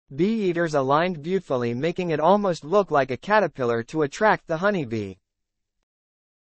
Bird🐦 bee eating shorts
Bee Eaters aligned beautfully making it almost look like a caterpillar to attract the honey bee